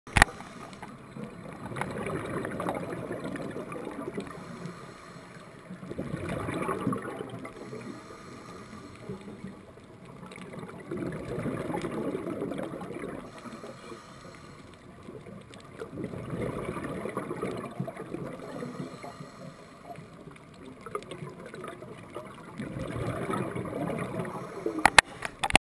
White Tip Reef Shark swims close
Barracuda Point Dive, Sipadan, Malaysia